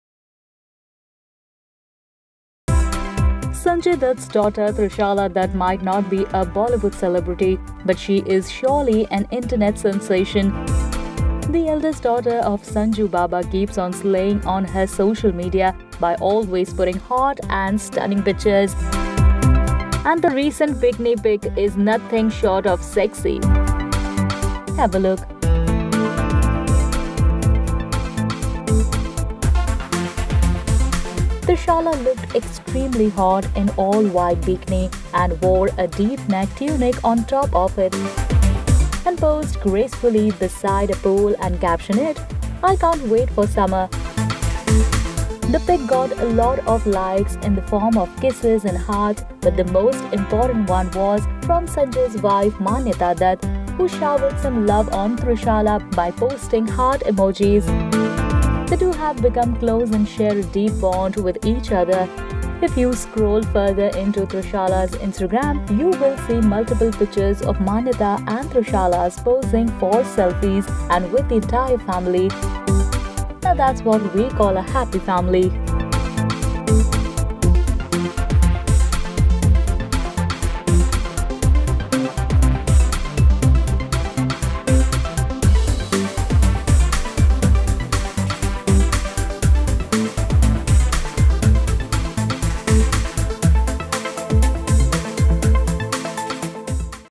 Sanjay Dutt's Daughter Bikini Pic Goes VIRAL And Maanayata Dutt Loves It! | LehrenTV
The eldest daughter of Sanju baba posted a hot bikini pic which got a big thumbs up from Sanjay's wife Maanayata! Have a look.